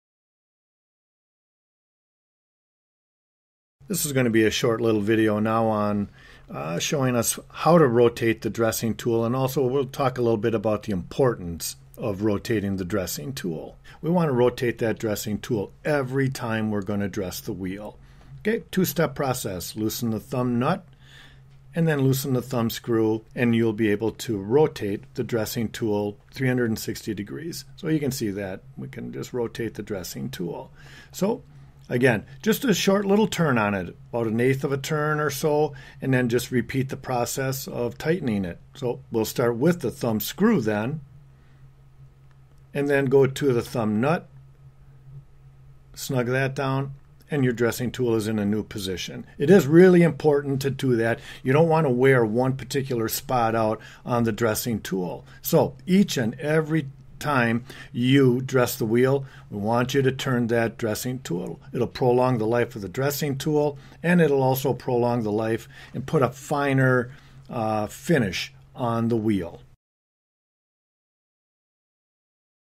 Rotating The Dressing Tool
A short clip on rotating the dressing tool on the E-ZSharp Machine.

E-Z Sharp Machines | The Pro's Choice
Skate Sharpening Equipment & Supplies